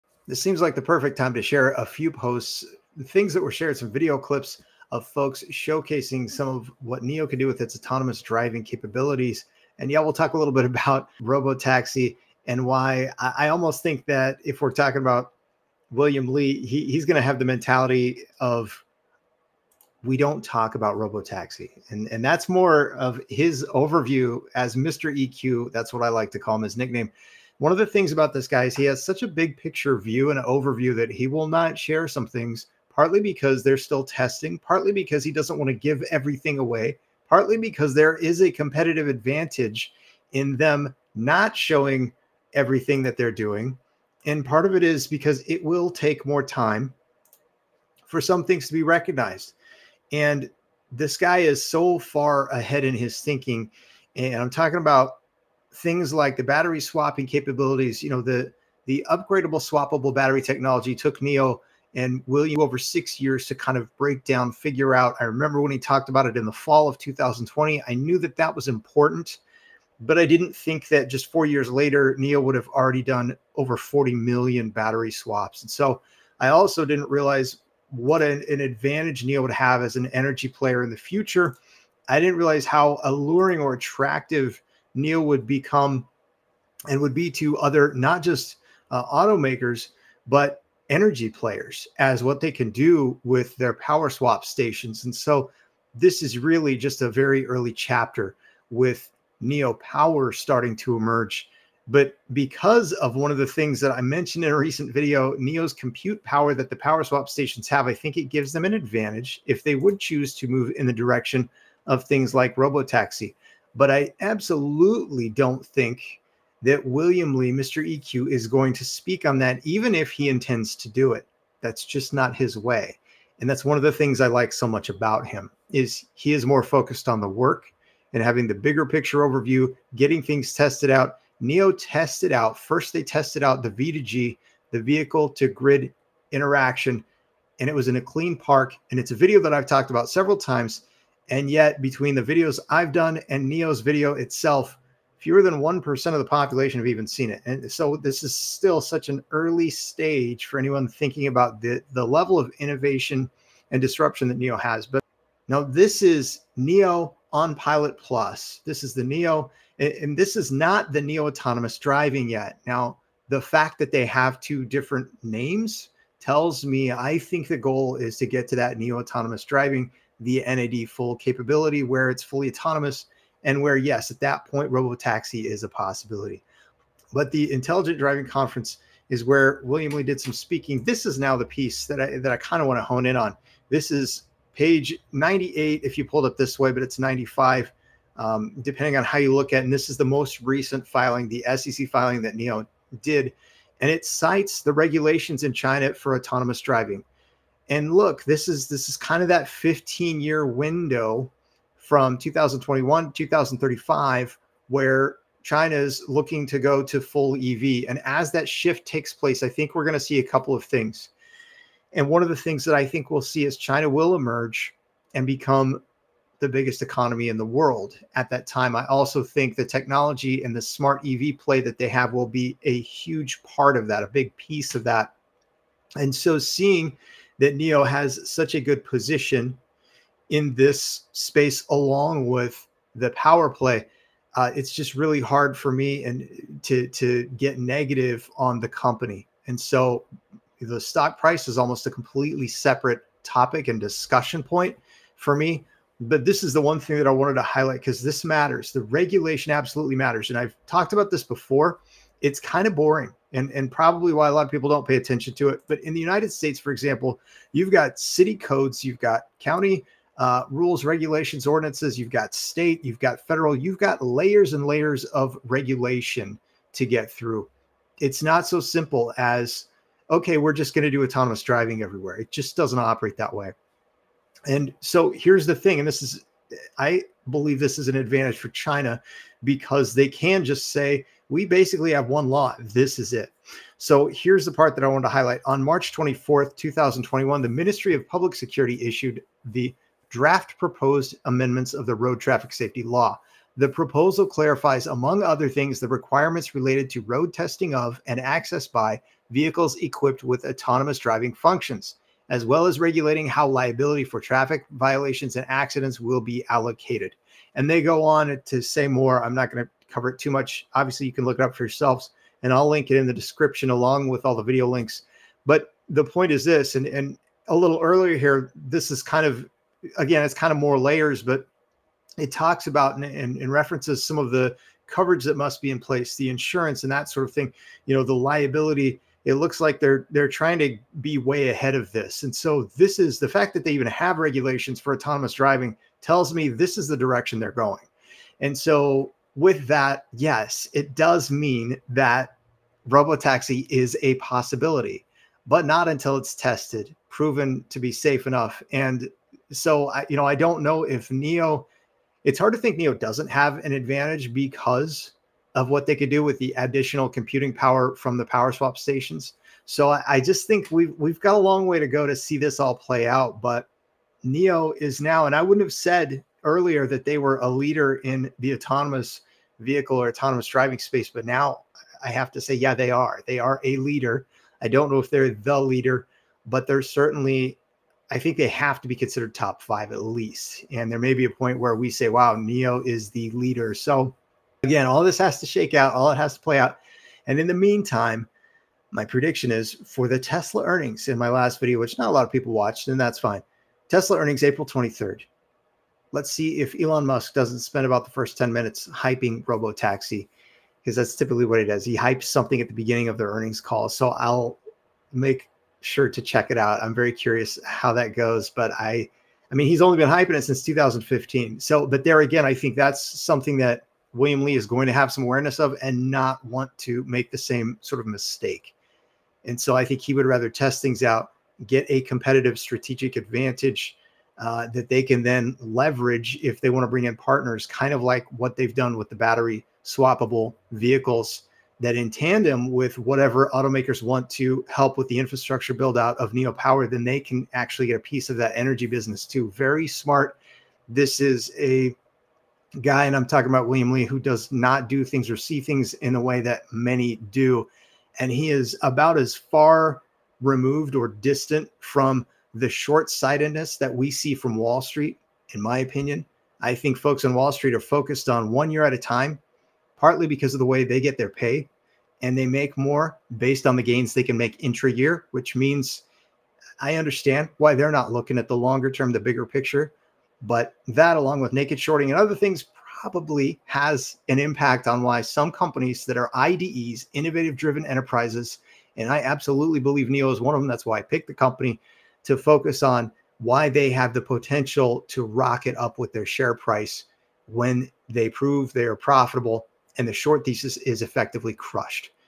NIO CEO William Li🤫"Don't Talk About Robotaxi!" 😉😂
I'm not an attorney or financial advisor. I ❤️🏀!
NIO video clips of autonomous driving & my thoughts on robotaxi potential and how regulations in China already have provisions for autonomous driving/robotaxi.